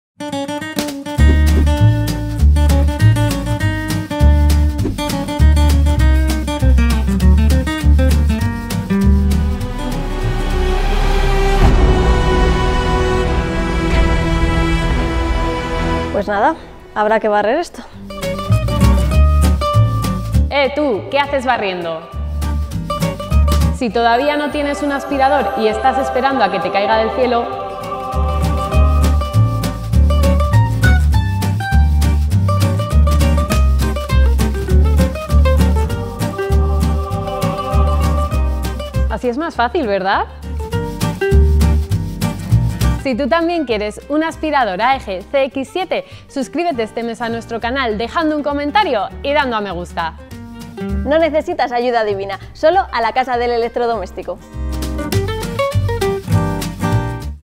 🎁🏡 Aspirador Aeg | Sorteo
SORTEO FINALIZADO

¡ENHORABUENA GANADORA!
🥇 Maty arma
Suplentes:
🥈 JL Rc
🥉 Nuritajojo
Válido para todo el territorio ESPAÑOL.

Características destacadas del aspirador:

- Aspirador escoba 2 en 1
- Funcionamiento sin cable
- Función de aspirador de mano
- Batería de litio de alta densidad (2100 mAh)
- Autonomía hasta 45 minutos
- Cepillo autolimpiante
- Luces LED frontales
- Doble filtro